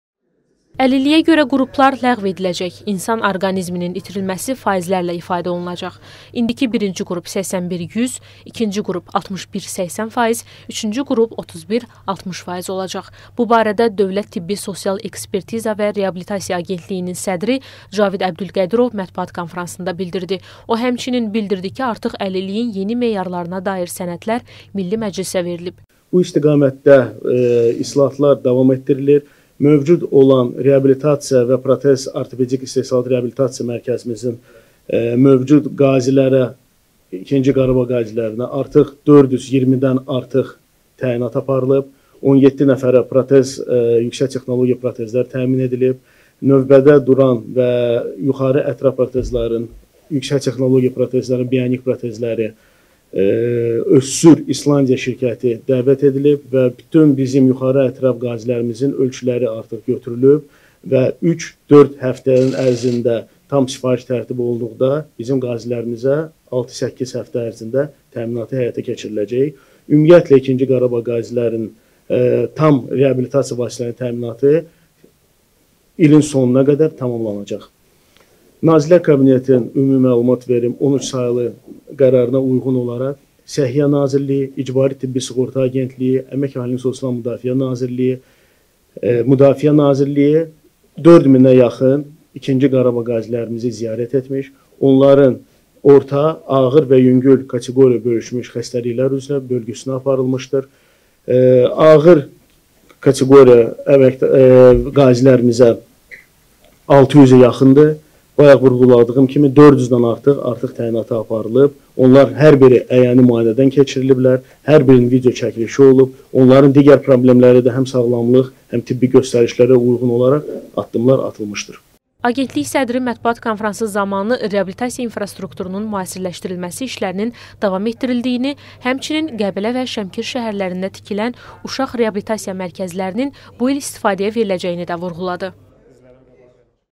Əlillik faizlərlə ifadə olunacaq
Əlilliyin yeni meyarlarına dair sənədlər Milli Məclisə təqdim olunub